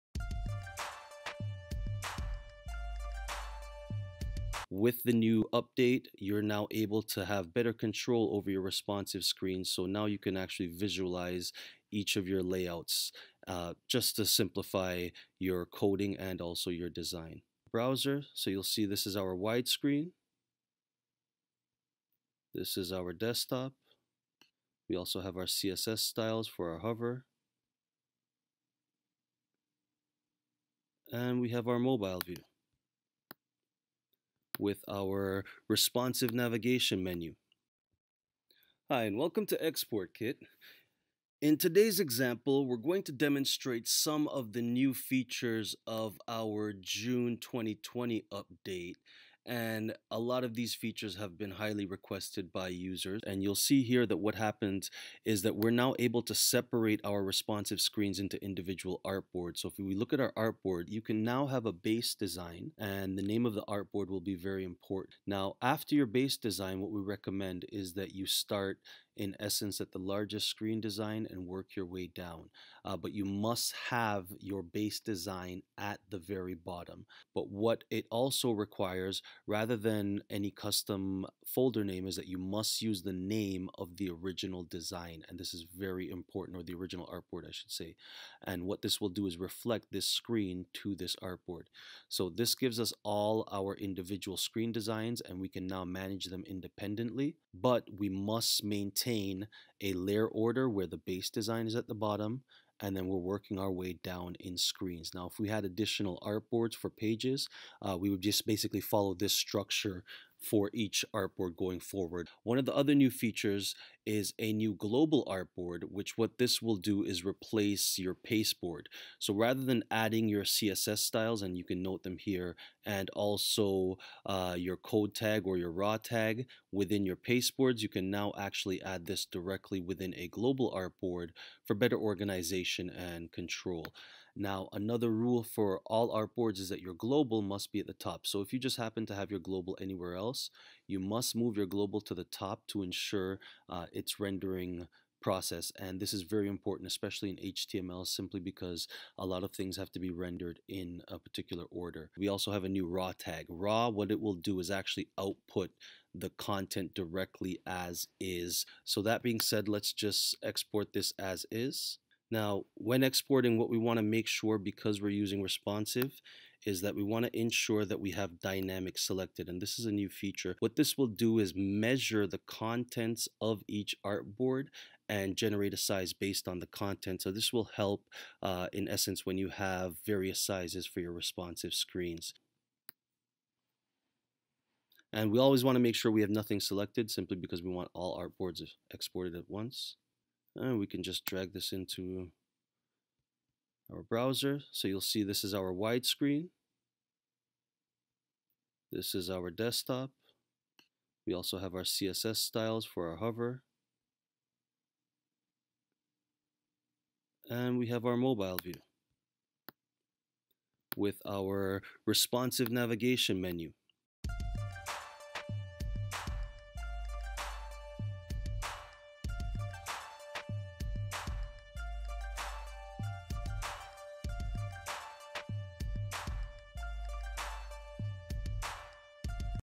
Responsive XD Artboards - June 2020 Update
The June 2020 update for Lightning Storm has many new features including better responsive support for Artboard screens, a new ${global} and ${raw} tag for better organization and more.

Get full access to HTML, JavaScript, WordPress, Android, iOS, Xamarin and more with your Business Subscription.